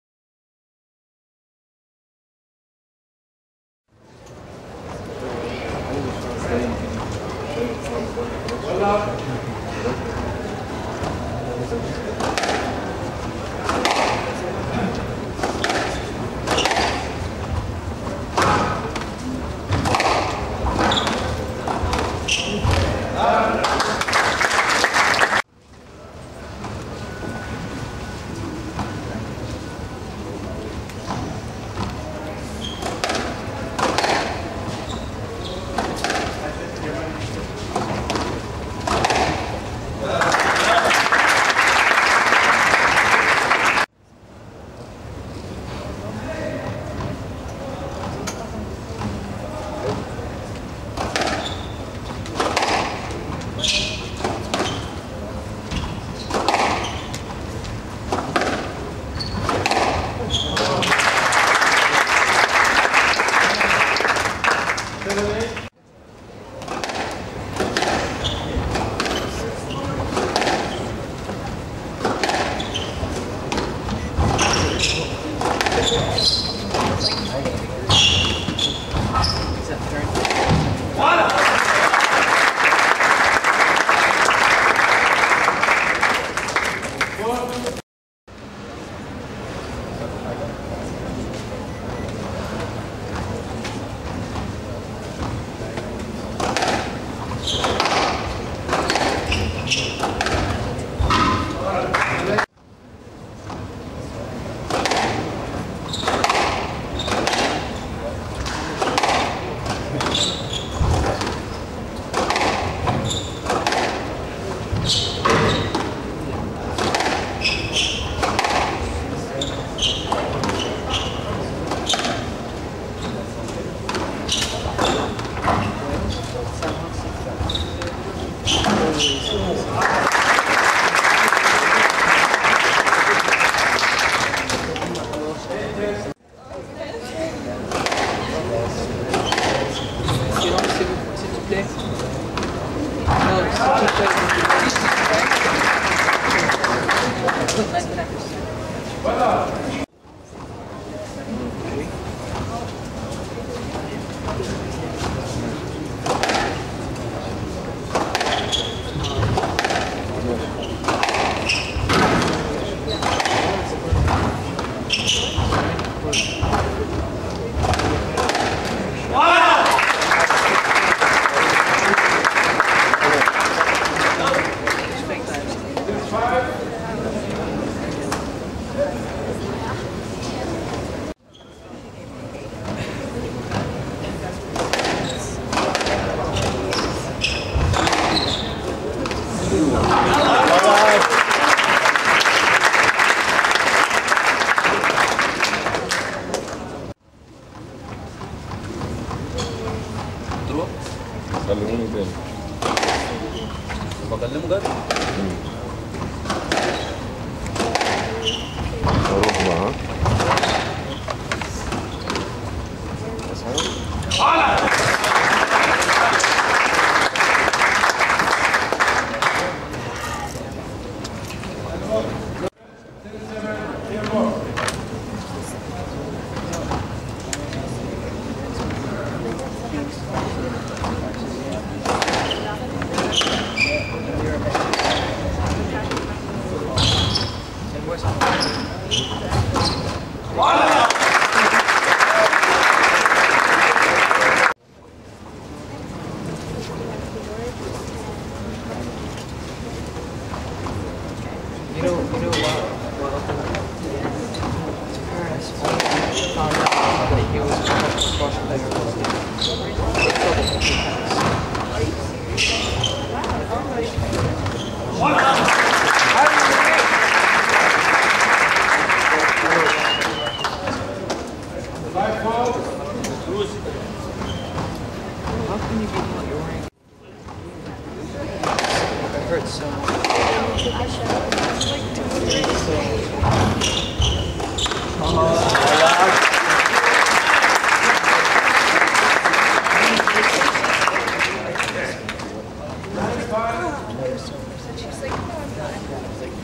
Nour el Sherbini best shots against Engy khairala in Atco Miro classic wispa final
Nour el sherbini playing on of her best matchs in her third wispa tour before wininig the world title the match ws against engy khairala the 29 years old while nour 13 years old her is her beast shots and rally in the match